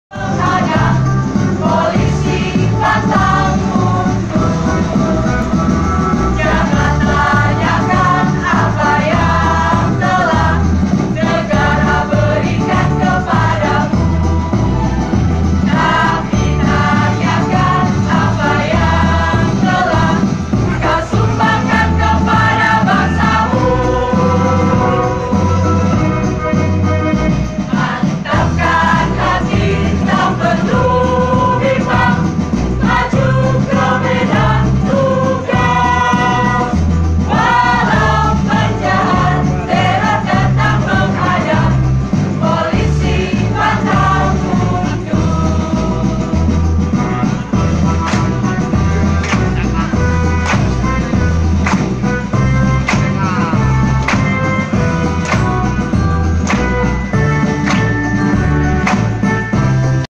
Paduan Suara Polres Wonosobo
Saat acara peresmian gedung baru Satpas SIM Polres Wonosobo (8/12/2016)